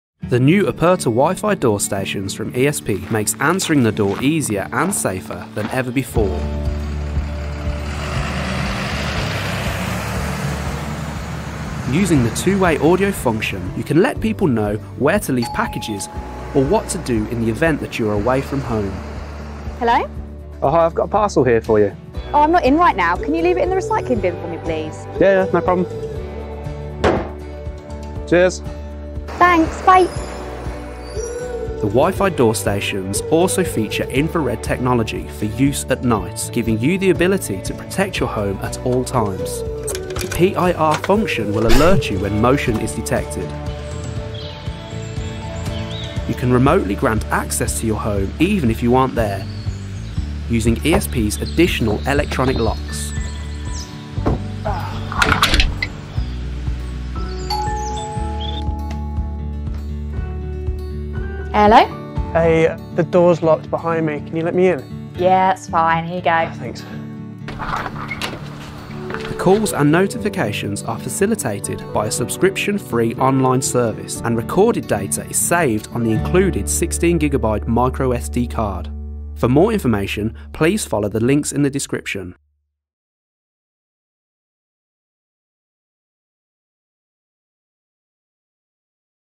ESP Aperta | IP PoE Door Stations, Video Monitors & WiFi Door Release Systems (No Subscription Fees)
ESP Aperta | Secure, stylish and reliable communication, developed today for tomorrow.

Discover a new range of IP PoE communication. Aperta IP intercoms provides a simple, stylish and reliable solution from planning, installation and use.
Powered by Click Smart+
Aperta IP products are all Click Smart+ compatible. Helping you to make smart solutions for a safer and more comfortable home.

Capacity to accommodate over 200 users while maintaining high performance. Perfect for large complexes.

- IP PoE Installation:- Save time with a quick and easy installation. Aperta IP stations are simply wired using Cat5E utilising Power over Ethernet technology.
- *Over 200 Users:-  Aperta Multi Way Door Station is perfect for large
apartment complexes and can accommodate over 200 users
- Lock Support:- ESP offer a range of compatible locks to control building access via the outdoor station, system monitor or the Click Smart+ app. (No subscription or ongoing usage fees apply)
- Impact Resistance:- In addition, the PoE multi way outdoor station has been
designed with high impact protection for extra durability.
- HD Video & Full Duplex Audio:- See and hear clearly with our HD Video and Full Duplex Audio. Watch live and also playback missed events.
- 7” HD Touch Screen Monitor:- Can support both Ethernet and WiFi for pairing to the Click Smart+ app allowing users to answer calls, and even unlock the door from their Smart Device.
- IP PoE 4+2 Port Switch:- An IP PoE Switch which supports up to 4 devices and 2 through ports, is supplied with each and every kit, making the installation an easy process.

Thanks to Aperta and ESP, protect your business or private property with a door entry IP65 HD Proximity Reader.

Never miss a thing with video recording and playback. Easy IP PoE Installation. NO SUBSCRIPTION REQUIRED

West Base Electronics Ltd T/A westbasedirect.com is an Authorised Stockist & Distributor of ESP, a Scolmore Group brand..

UK Delivery Only